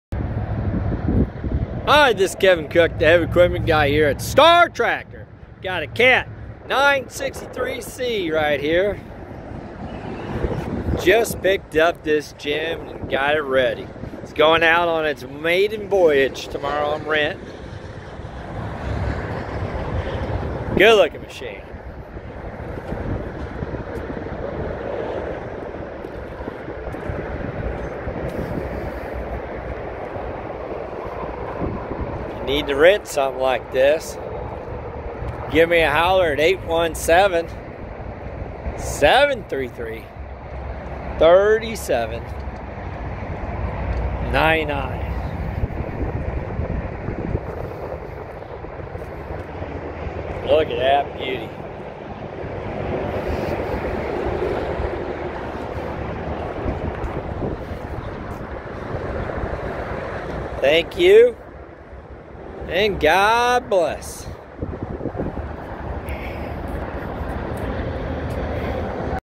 Caterpillar 963C Crawler Loader for Rent
This Caterpillar Crawler Loader is going out for Rent at Star Tractor.

Cat 963C Track Loader for Rent